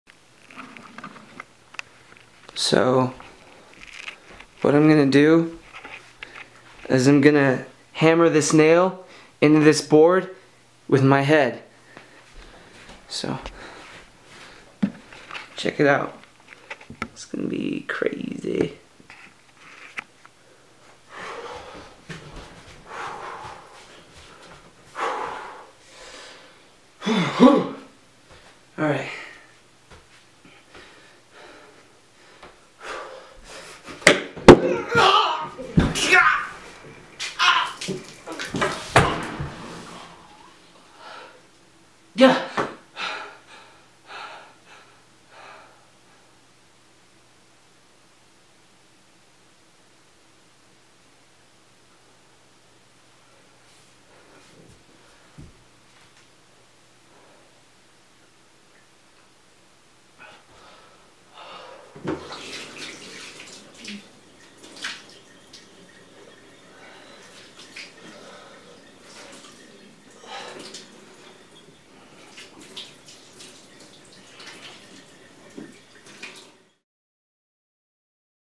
Stupid guy hammers nail with his head
My friend tried to hammer a nail in with his head because he saw some guy do it on Stan Lees Superhuman's show. He failed.